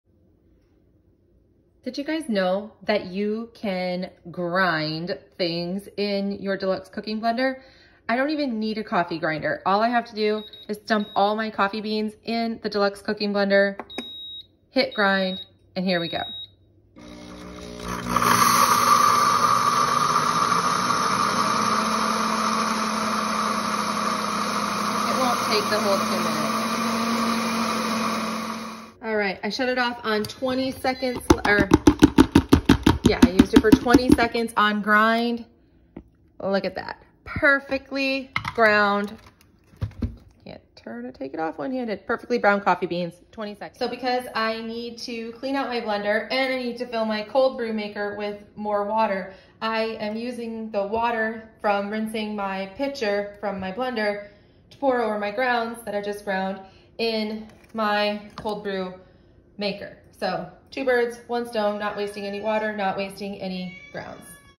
Grinding Coffee in the Deluxe Cooking Blender
Here is how you can grind an entire bag of coffee in about a minute in your Pampered Chef Deluxe Cooking Blender.